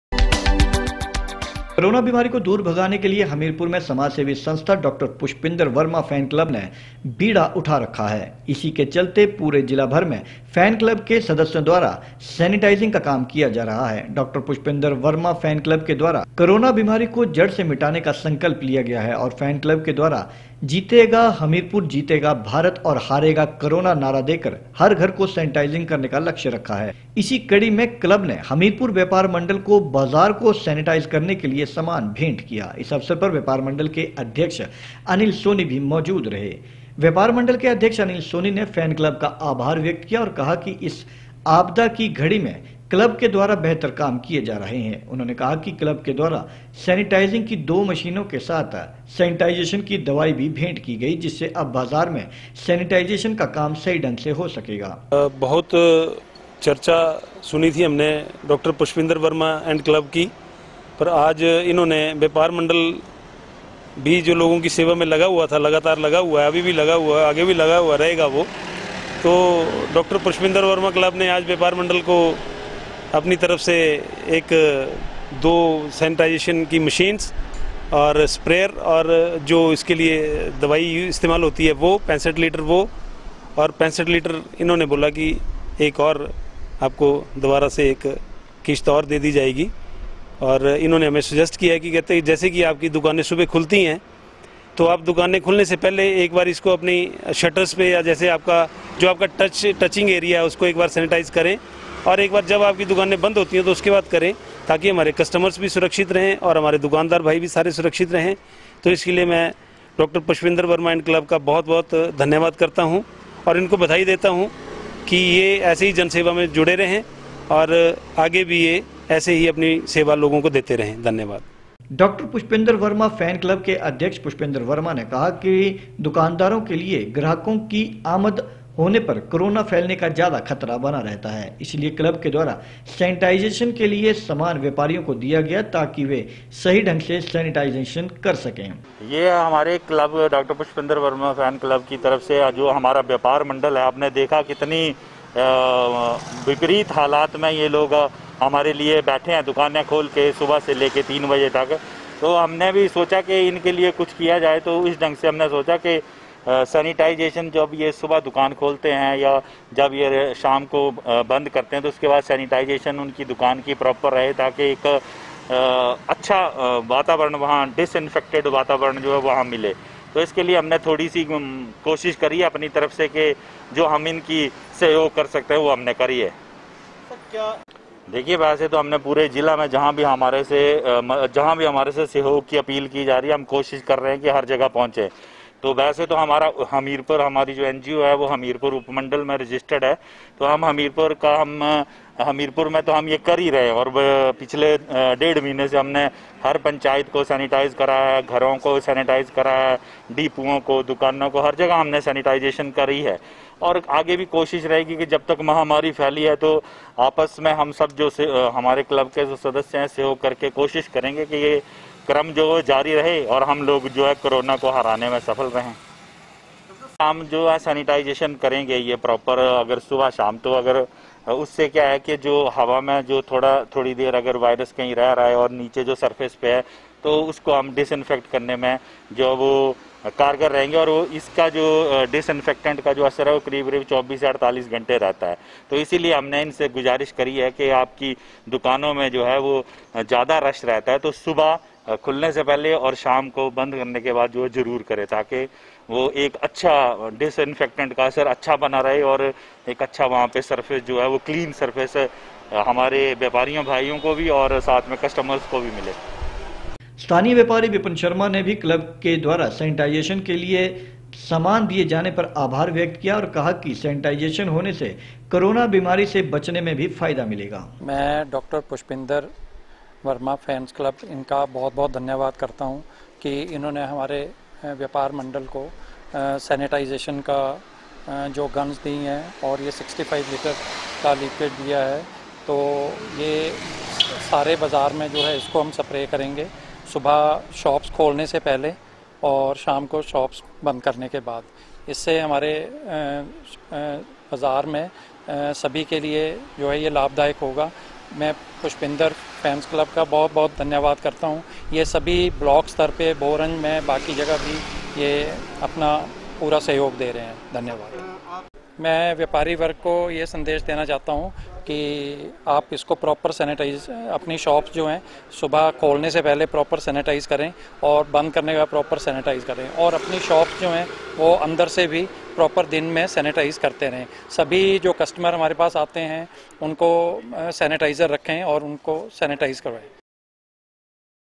14 may 13 जीतेगा हमीरपुर,जीतेगा भारत और हारेगा कारोना नारा देकर हर घर को सेनेटाइज करने का लक्ष्य रखा
कोरोना बीमारी को दूर भगाने के लिए हमीरपुर में समाज सेवी संस्था डा पुष्पेन्द्र वर्मा फैन क्लब ने बीडा उठाया हुआ है और इसी केचलते पूरे जिला भर में फैन क्लब के सदस्यों द्वारा सेनेटाइजेशन का काम किया जा रहा है। डा पुष्पेन्द्र वर्मा फैन क्लब  के द्वारा कोरोना बीमारी को जड से मिटाने का संकल्प लिया है और फैन क्लब के द्वारा जीतेगा हमीरपुर, जीतेगा भारत और हारेगा कारोना नारा देकर हर घर को सेनेटाइज करने का लक्ष्य रखा है। इसी कडी में क्लब ने हमीरपुर व्यापार मंडल को बाजार को सेनेटाइज करने के लिए सामान भेंट किया। इस अवसर पर व्यापार मंडल हमीरपुर के अध्यक्ष अनिल सोनी भी मौजूद रहे।